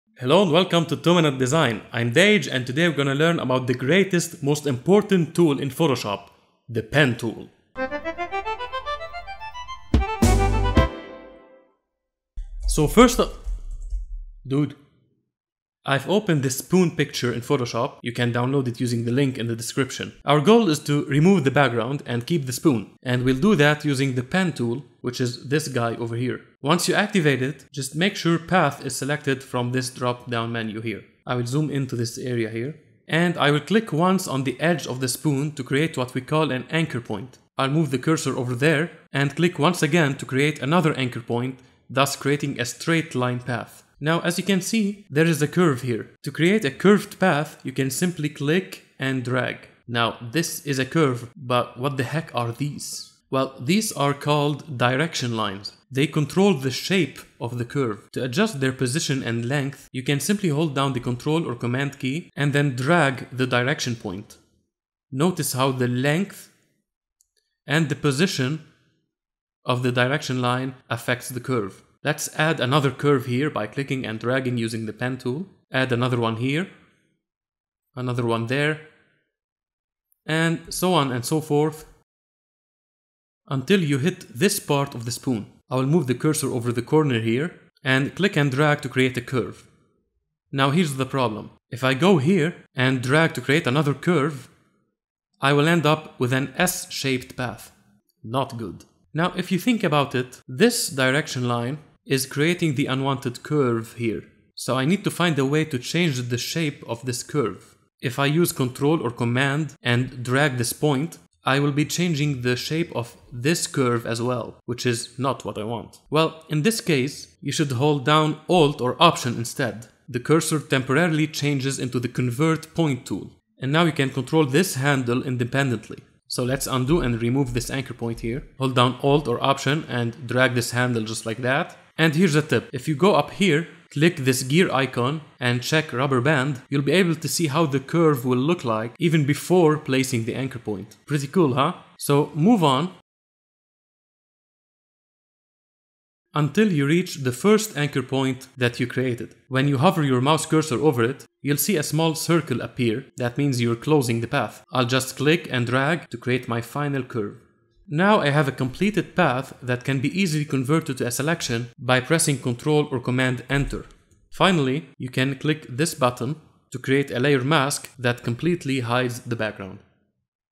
Photoshop's Pen Tool
The Pen tool is Photoshop's greatest, most accurate tool. Today we will learn how to create a path using the Pen tool, which will allow us to hide the background and keep the traced object.

Along the way, we will learn about anchor points, direction lines, straight and curved lines, as well as some awesome tips and tricks.